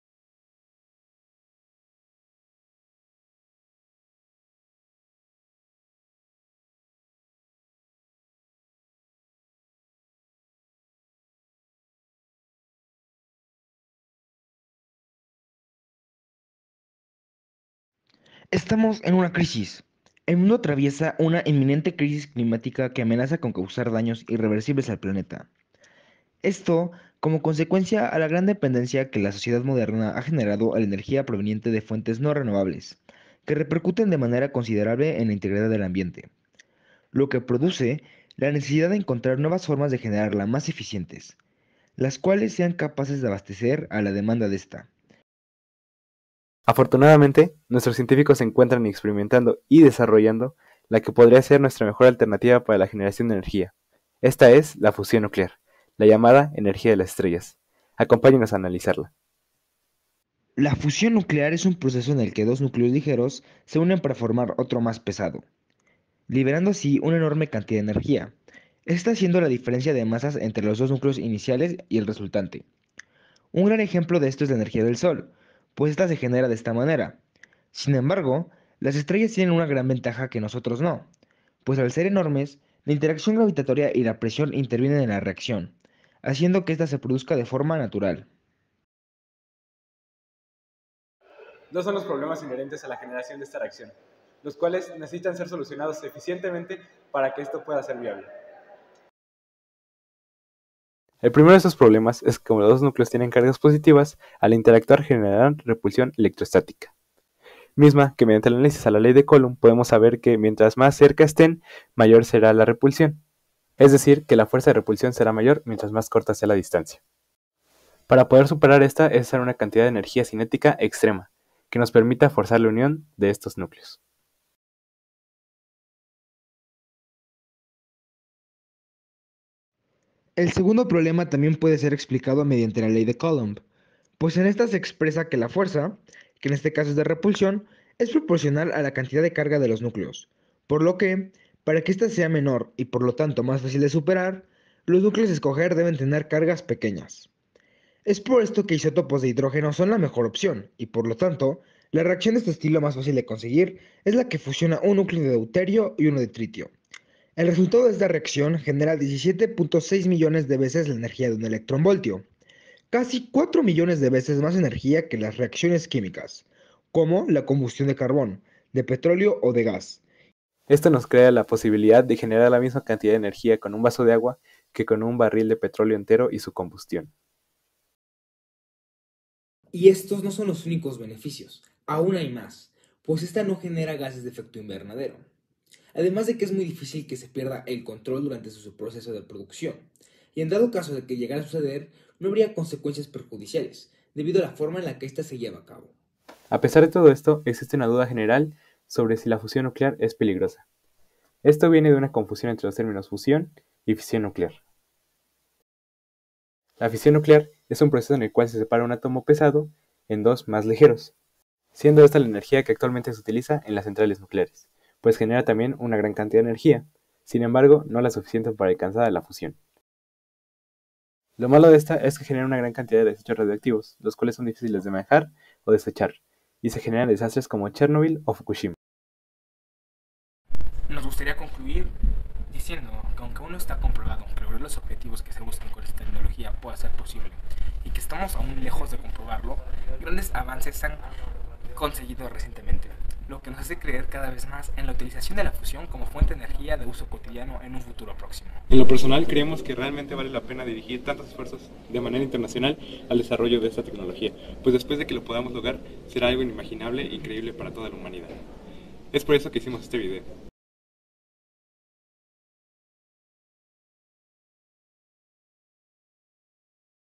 Fusión nuclear: "La energía de las estrellas"
Concursos Interpreparatorianos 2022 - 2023. Video de divulgación científica
Video finalista